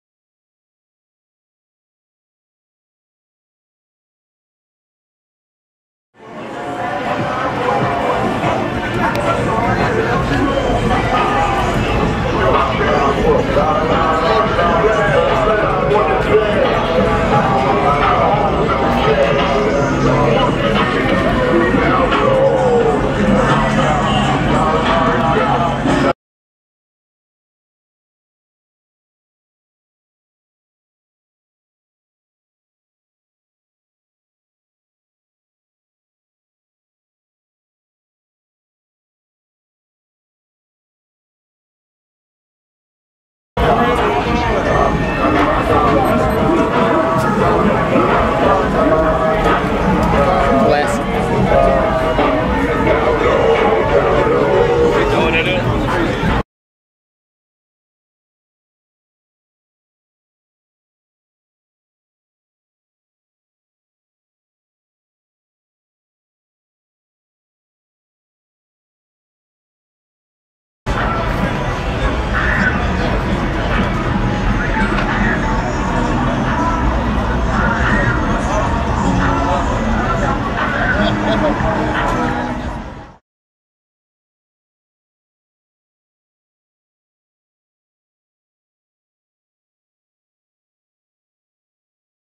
Chevrolet StepSide C-10 on 28" Forgiato Grano Wheels (Short Bed Scottsdale 28s)
Here is a really dope C-10 I ran across after the Linny J carshow in Tampa, FL. You don't see too many of these stepside C-10s done up in the big rim game!!! This truck is super clean and is sitting on a nice set of 28" Forgiato Grano wheels.